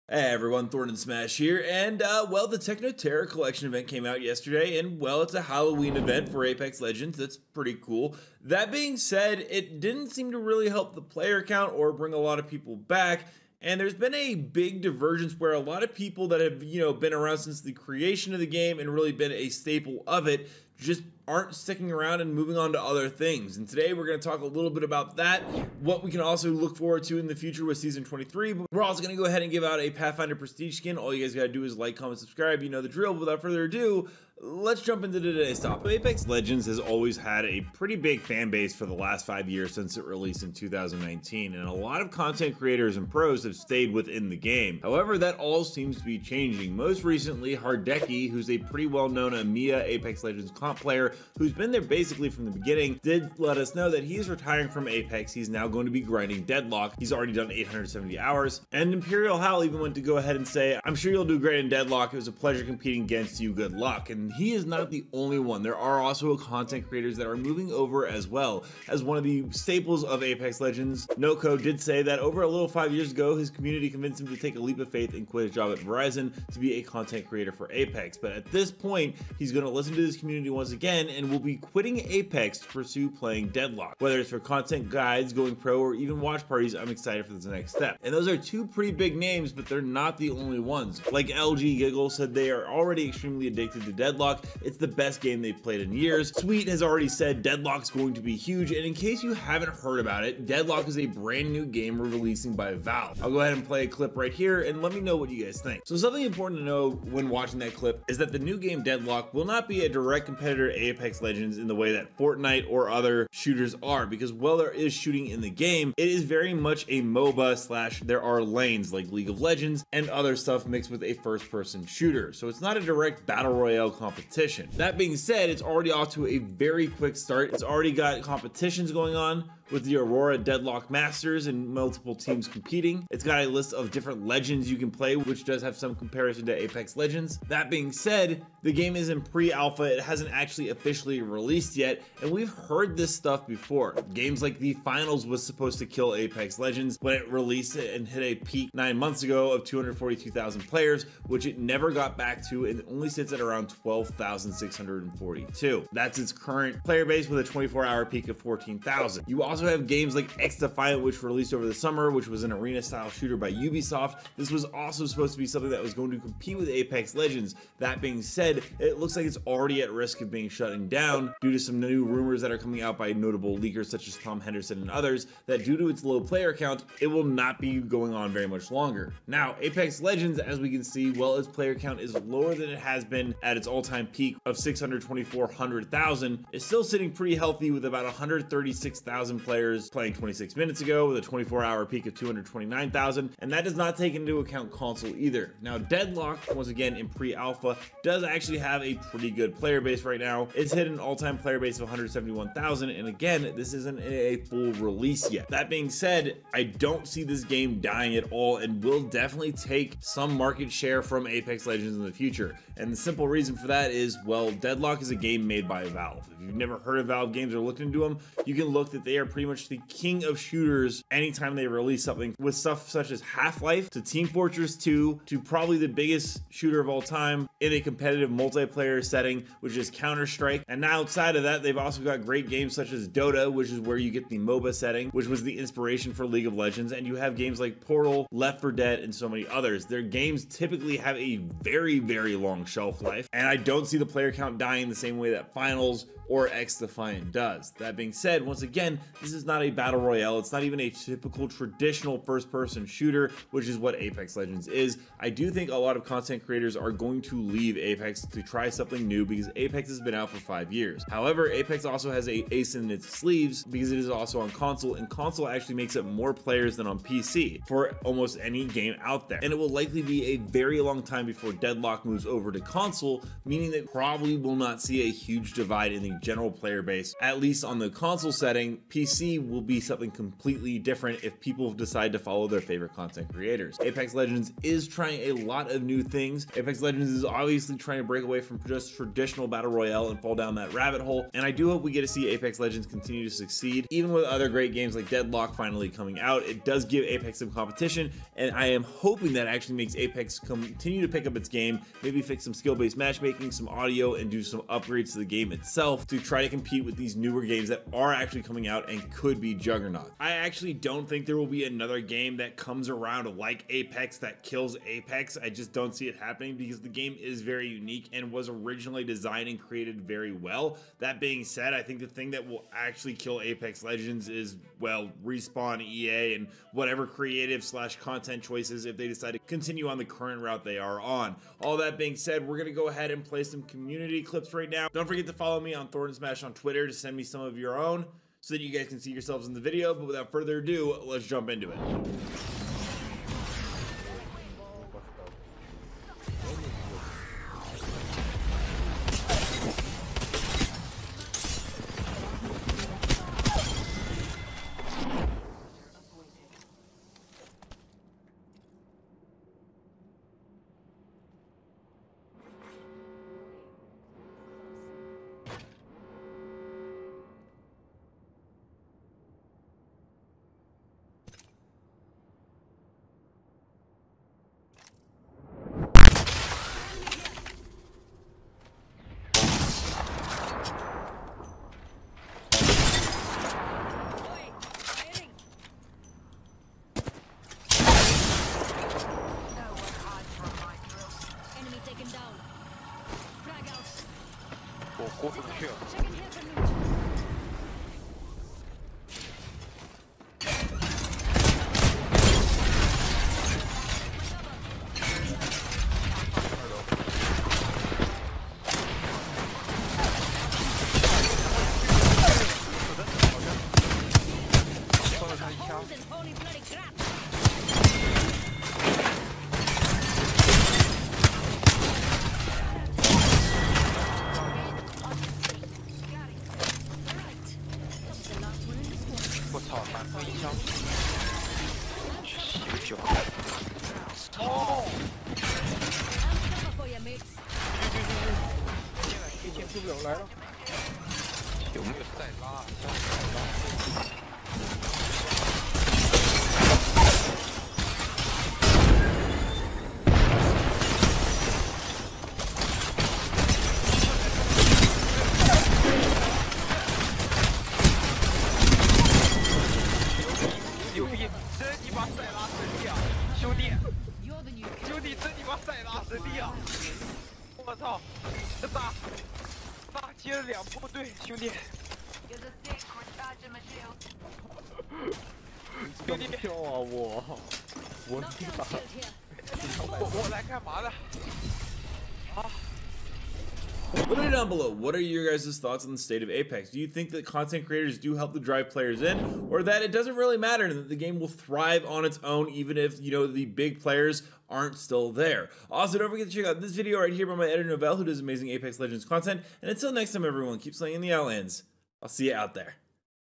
Apex Legends New Collection Event & Update Wasn't Enough
Apex Legends New Event & Content Wasn't Enough as Apex Legends, a hit battle royale, fell short of EA's financial expectations. Critics point to issues like unbalanced gameplay, lack of fresh content, and excessive monetization as reasons. This could be due to EA prioritizing short-term profits over long-term player engagement.
 Ultimately, only time will tell what impact EA's business practices in China will have on Apex Legends.
Credit To Dataminers:
Shrugtal: